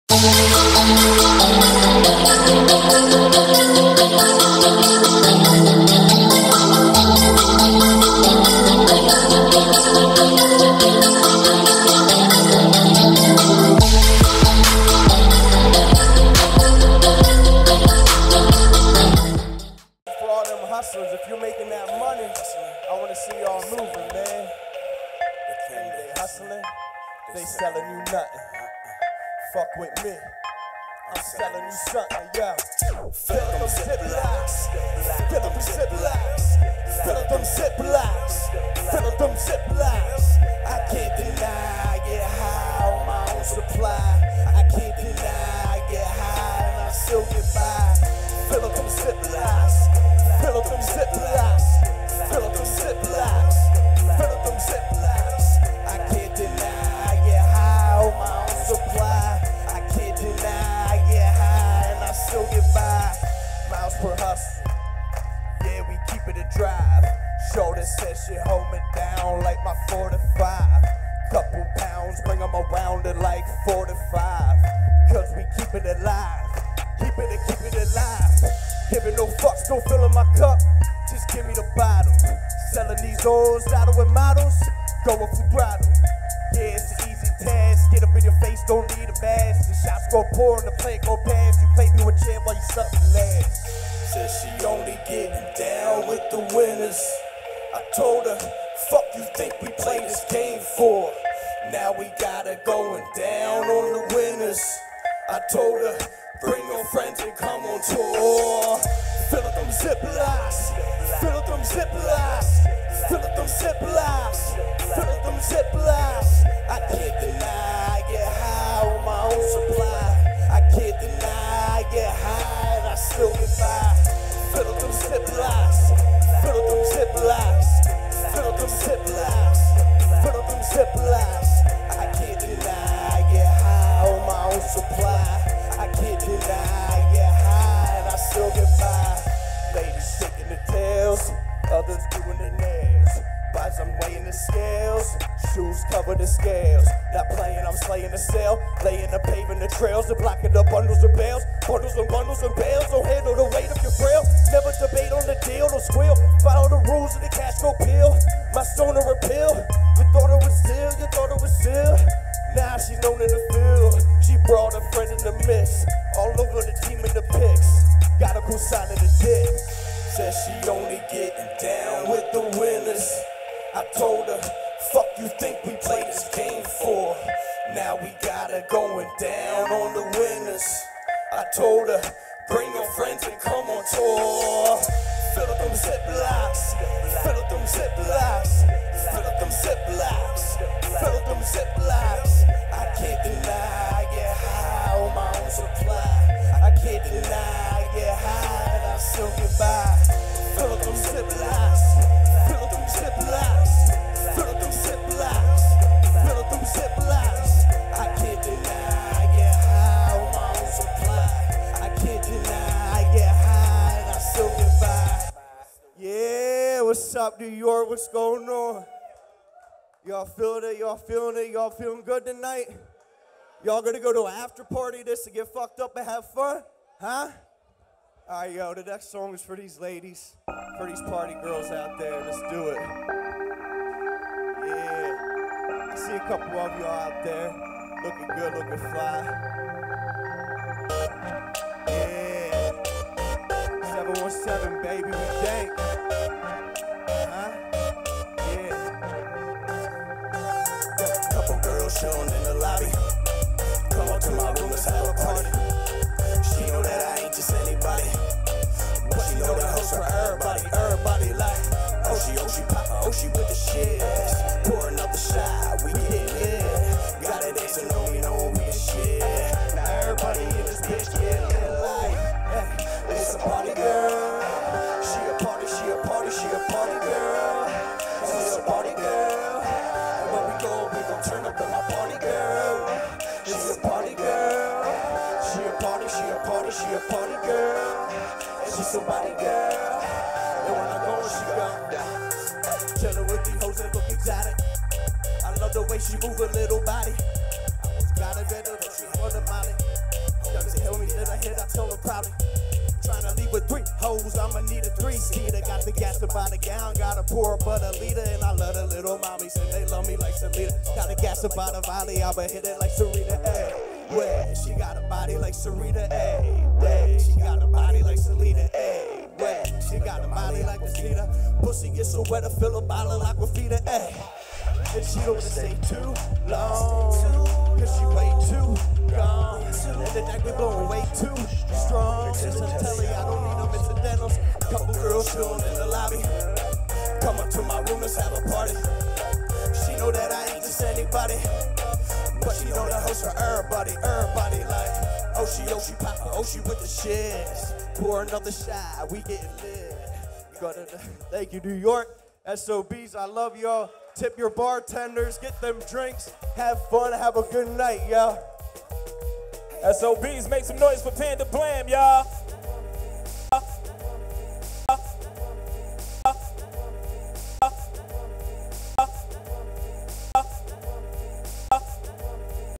PANDA BLAM - MAY 31ST 2016 FACES IN THE CROWD SHOWCASE @ SOB'S
May 31st 2016 "Panda Blam" @PandaBlam performed in front of a packed audience at Faces In The Crowd Showcase at SOB's NYC.

to learn more about our showcase.

Faces In The Crowd Showcase strives to give a platform to Talented Artist.

Performance videos shot and edited by @degudlife